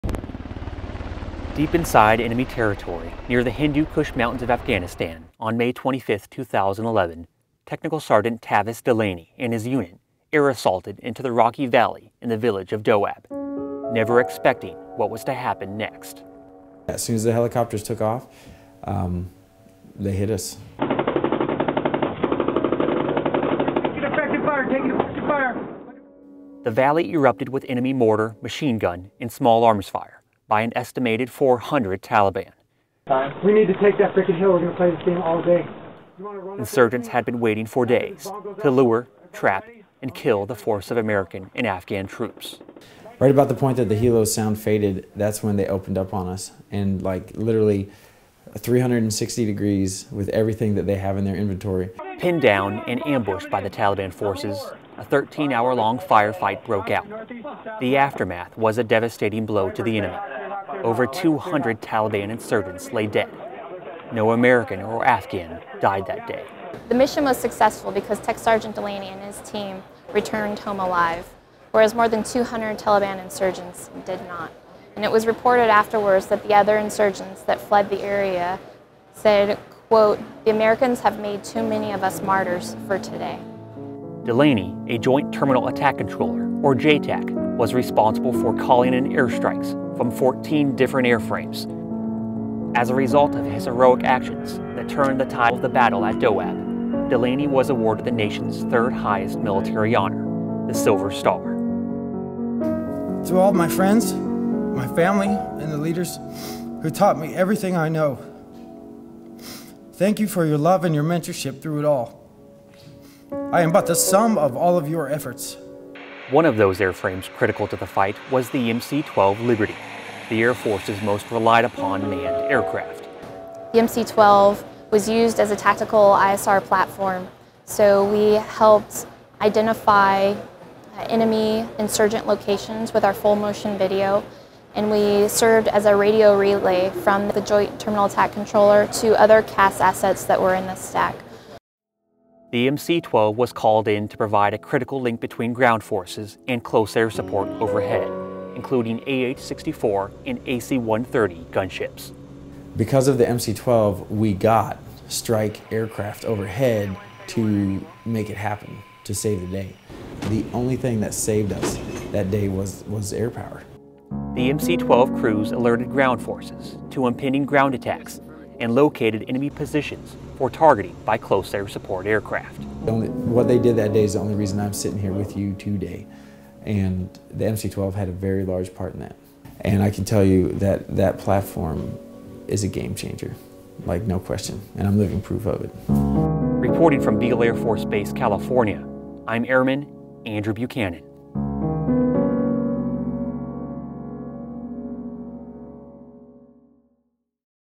MC-12 Plays Critical Role in Afghanistan Battle
The MC-12 Liberty played a critical role in establishing a link between ground forces and close air support aircraft during the battle of Do Ab in Afghanistan on May 25, 2011. Tech Sgt Tavis Delaney was responsible for communication with the aircraft and calling in the support in the face of danger. As a result of his heroic actions, Tech. Sgt. Tavis Delaney was awarded the Silver Star.